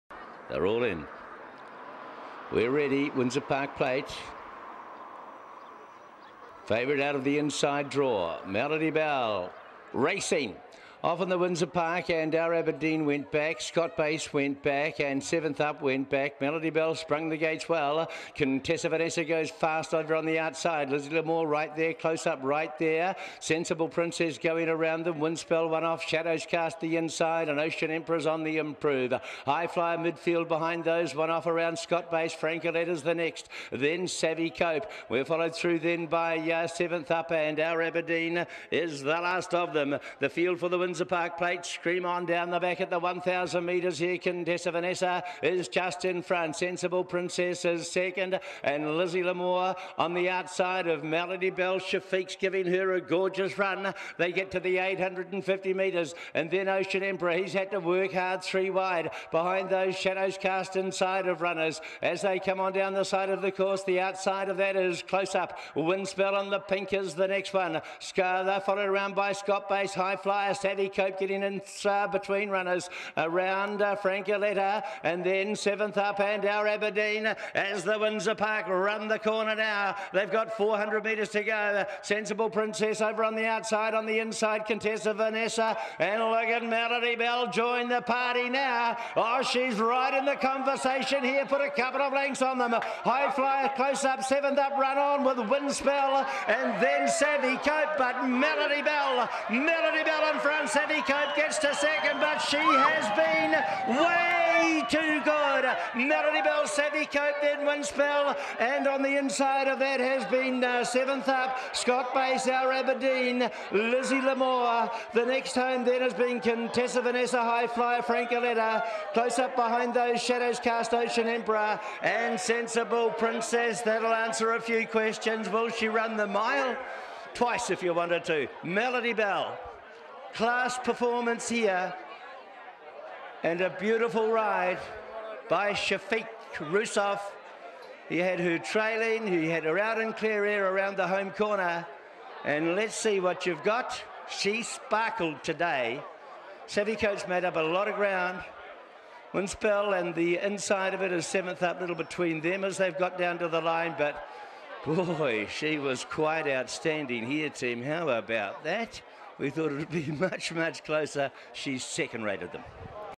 2018 Group 1 Windsor Park Plate: Melody Belle
The 2018 running of the Group 1 Windsor Park Plate.

1st Melody Belle $3.40 & $1.70
2nd Savvy Coup $2.40
3rd Wyndspelle $4.60
Quinella $13.90, Trifecta $298.10, First4 $5,325.10
Time: 1:35.39